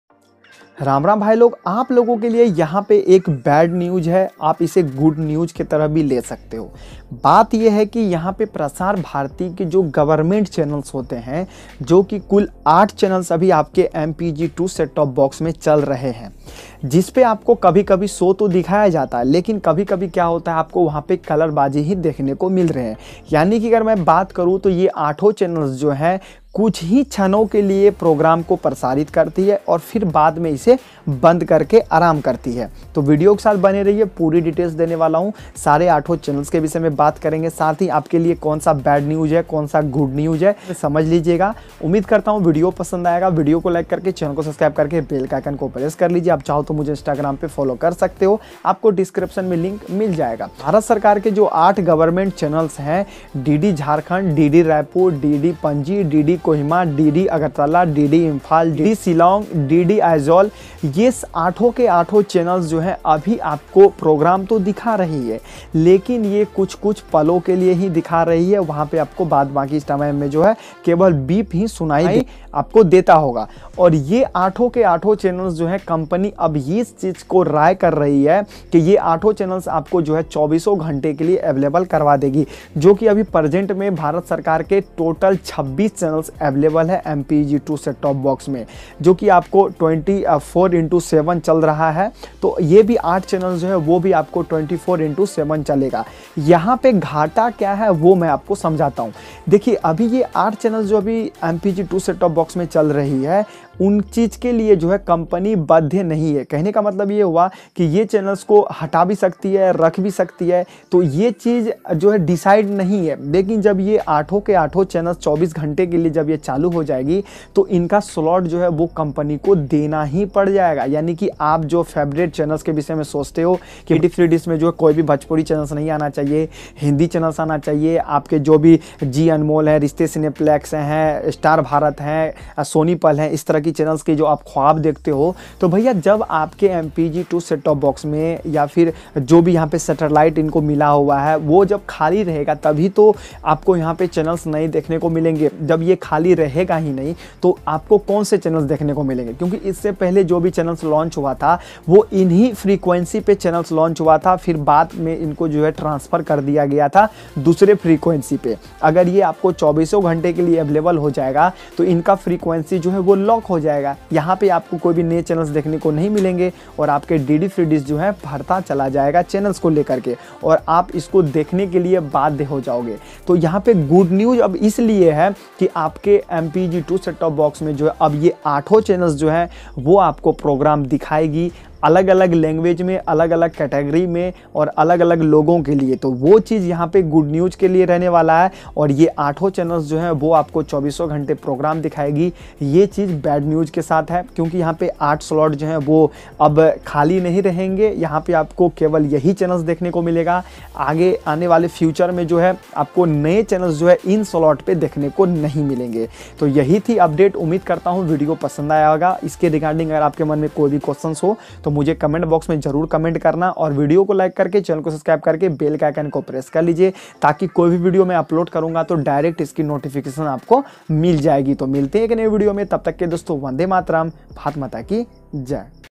Prasar Bharati 8 Channel 24×7 | 45 Eauction MPEG 4 Box | Government channel
Dear family, aaj ki video main aapko bataya hun prasar Bharati ke government 8 channel Jo aapke DD free Dish mein chal rahe hain vah ab 24 into 7 rahane wala hai Puri details video mein hai,

👉Ask your questions Type👇
                      🙏 । शुक्रिया । 🙏